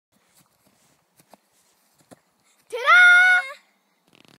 Cornfield Cartwheel
Emily La Rue cartwheels on queue at "Taildragger Lane" after her ride in Grandpa's Aeronca Champ.